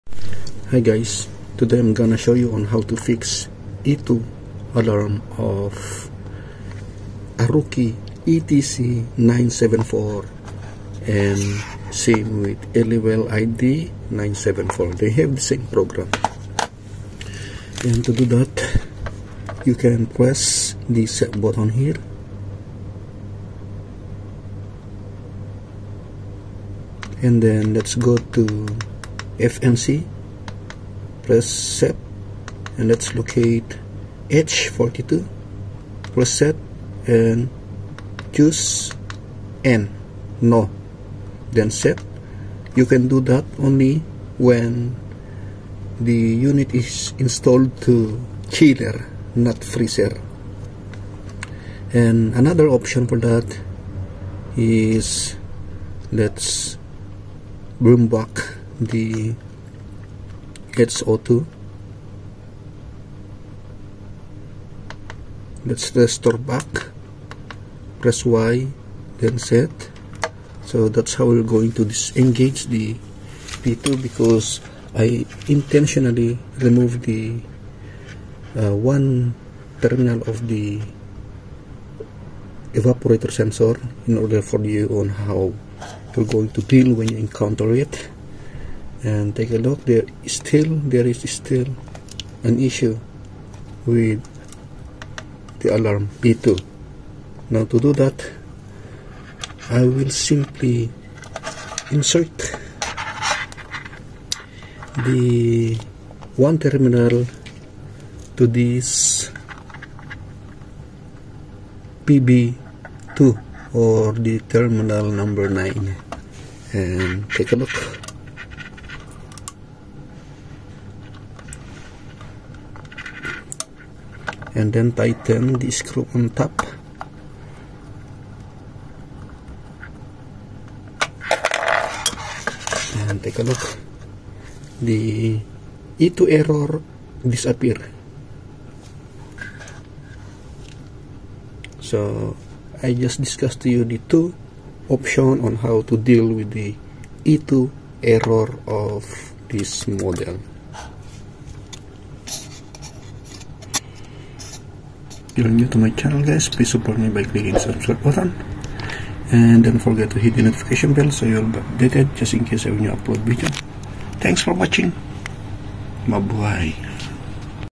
HOW TO FIX E2 ALARM OF ARUKI ETC-974 / ELIWELL ID974
A guide on how to get rid off to E2 alarm of aruki ETC 974 digital thermostat of commercial refrigerator